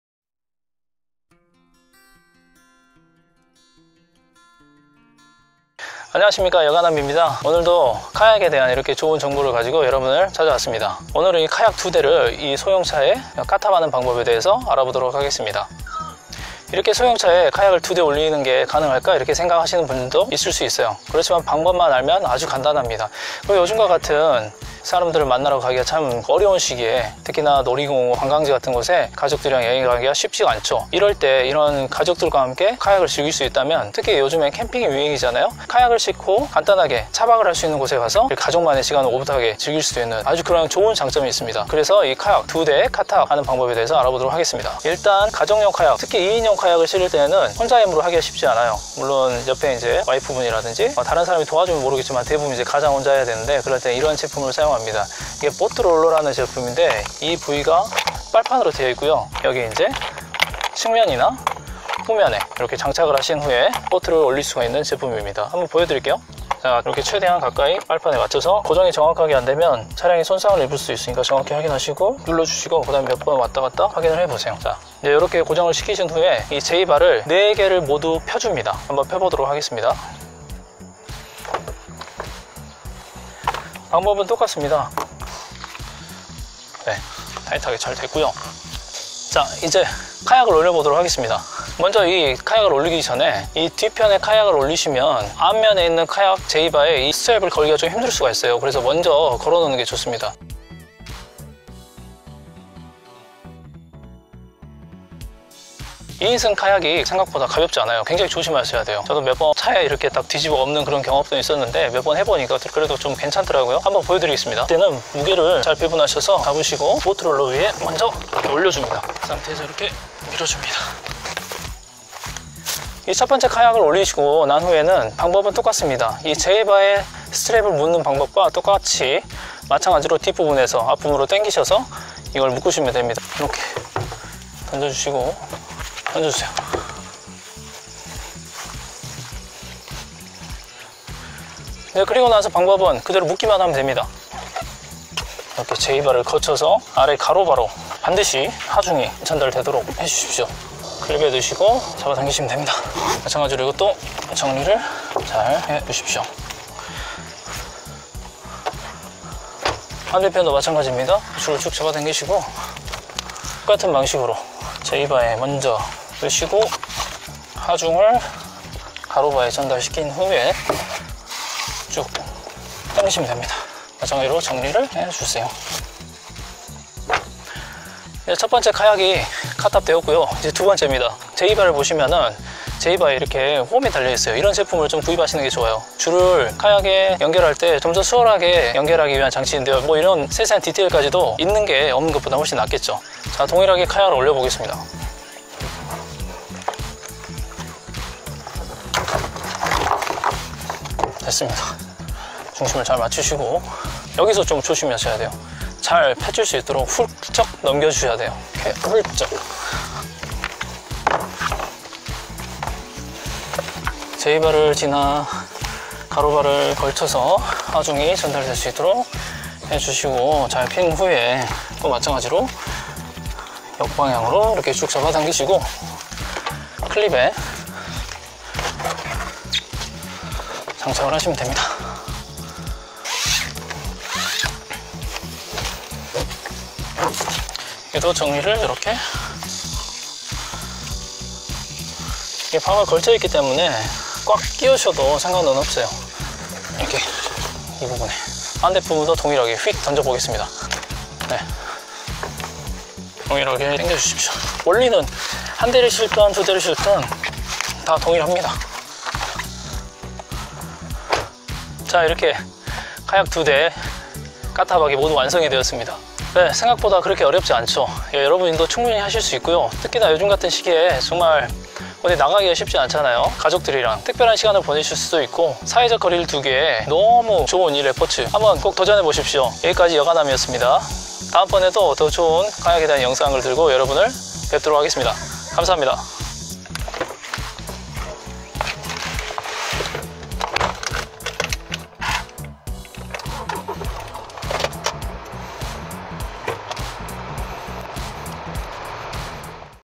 카약 두대 카탑하기 소형차에 가능할까?!
가로바 없이 카탑을 가능하게 해주는 소프트 루프랙

위 링크들로 들어가면 다양한 카약 용품을 만나실수 있습니다.
본인에게 가장 맞는 용품을 검색해서 구입하시길 추천 드립니다.

안녕하세요~ 여행 가는 남자 여가남입니다.

오늘은 차량에 카약을 두 대 올리는 방법에 대해 설명해 드리려 합니다.

보통 카약 혹은 카누를 차량 지붕위에 올리는 것을 카탑 한다고 합니다.

두 대를 자동차 지붕에 올리기 위해서는 이런 특수한 카약 캐리어가 필요합니다.
보통 J바 (제이바)라고 불리는 카약캐리어를 이용합니다.

J바의 경우 국내에서 직접 구입하실 수도 있지만 알리익스프레스나 아마존에서 대략 20만원 중반에서 구입 하는게 훨씬 가격이 저렴합니다.
알리익스프레스나 아마존에서 검색해 보시면 매우 다양한 제품들을 보실 수 있습니다.
혹시 가격 때문에 양방향 J바(2쌍의 J바가 양방향으로 연결된 형태의 캐리어)를 선택하실수도 있는데, 제가 사용해 본 결과, 혼자서 이 양방향 J 바에 카약을 두 대 올리는 것은 거의 불가능했습니다. 무게 중심 맞추기가 굉장히 힘들어서 결국 중고로 판매했는데 저는 개인적으로 이 제품은 두 대를 운용하시는 분들에게 추천하지 않습니다.
가장 좋은 방법은 J바 두쌍을 구입하셔서 J바 한쌍당 한 대의 카약을 올린 다는 개념으로 접근하시는것입니다. 실제로 저도 이렇게 카약을 운반하고 있습니다.

카약을 카탑할때에는 각 4개의 J바 를 먼저 약 45`의 각도로 펴줍니다. 그리고 한 대의 카약을 먼저 올립니다. 제 카약은 2인승이여서 크기와 무게가 상당하기 때문에 Boat Roller (보트롤러) 라는 이런 장치를 사용합니다. 이 장치를 후면에 고정하여 카약을 기대어 자연스럽게 J 바에 에 올려 줍니다.

이 Boat Roller(보트 롤러)도 알리익스프레스에서 약 6만원 정도에 구입하실수 있습니다.

그 후에 카약을 J바의 중간에 오게 하여 중심을 잘 잡고 카약 스트랩을 단단히 고정시킵니다.

그리고 두 번째 카약을 동일하게 J바에 올린 후에 스트랩을 카약캐리어에 단단히 고정시켜 줍니다.

안전이 가장 중요하기 때문에 카약 두대의 전면과 후면에 다른 스트랩을 고정하여 차량에 설치되어 있는 견인 고리에 고정시켜주시는게 더 좋습니다. 이렇게 카약 전후에 스트랩을 고정하여 차량에 연결하는 것을 보통 타이다운이라고 합니다.
장거리를 이동하실 때에는 필수적으로 타이다운을 하셔야 하고, 단거리를 이동하시더라도 타이다운을 하시는게 심리적으로도 안정감 있고, 실제로도 스트랩 2개로만 고정한 것 보다 훨씬 안전합니다.

그 외에 안전을 위해서 몇가지 말씀드자면,
첫 번째, 카약을 실은 후 차량의 거동을 급하게 하지 않는 것이 좋습니다. 아무리 카약을 잘 고정하여도 돌발 상황이 있을 수 있으므로 차분하게 차량을 운행하시는 것이 좋습니다.
두 번째, 차량 속도는 80킬로 이하로 운행하시는 것을 권해 드립니다.
몇몇 동호인들 중에서는 카탑 후 100킬로 이상으로 운전했다고 자랑 아닌 자랑을 하시는 분이 있는데 좋아 보이지 않습니다. 무엇보다도 안전이 최우선입니다!

오늘은 카약 두 대를  차량에 올리는 방법에 대해 알려 드렸습니다.

카약을 두 대 운용했을 때에는 가족과 함께 카약을 즐길 수 있다는 장점이 있습니다.
단! 카약을 올리고 내리는 것은 본인이 하셔야 가족들이 자주 따라옵니다. 하~~~ (한숨)

가장들 힘내십시요!

다음 시간에 뵙겠습니다.
여가남 이었습니다.
감사합니다!